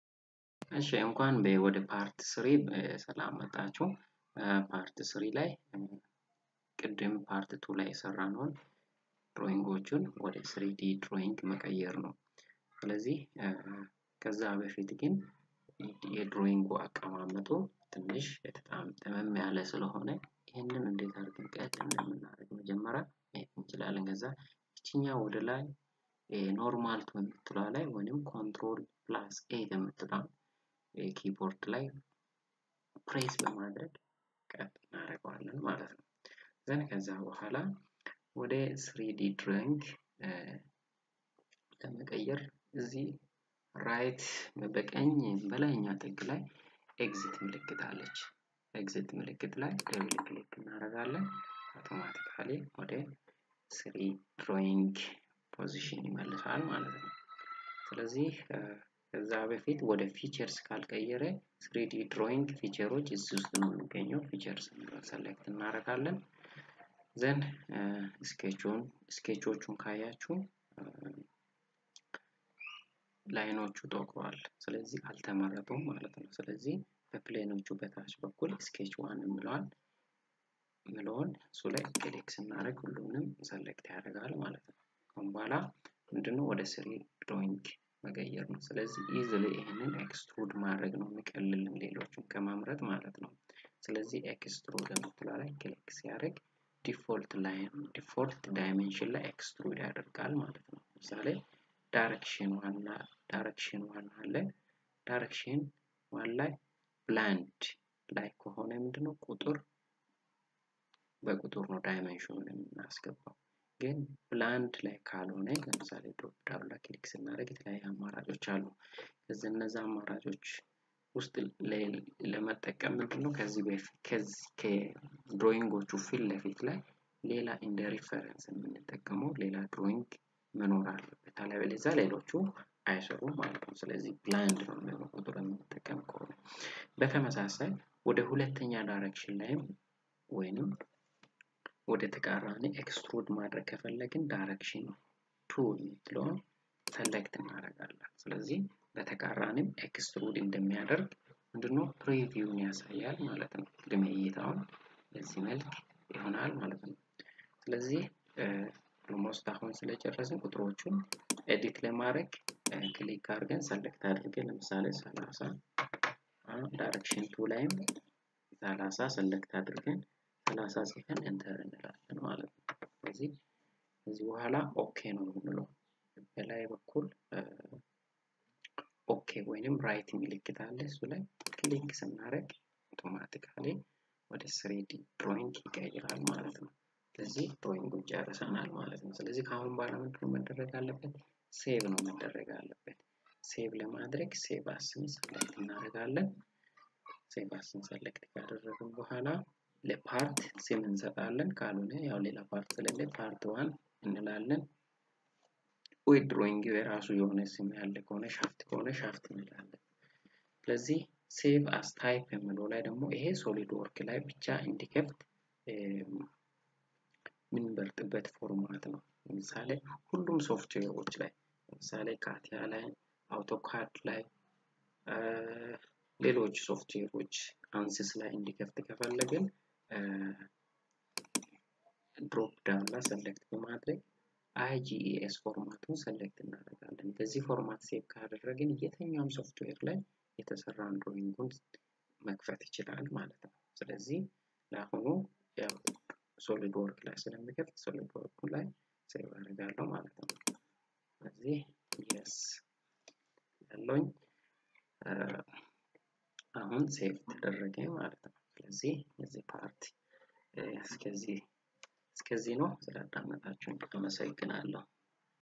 SOLIDWORK in Amahari, ለጀማሮች how to convert 2D into 3D by using extrude feature ክፍል 3
SOLIDWORK Tutorial in amharic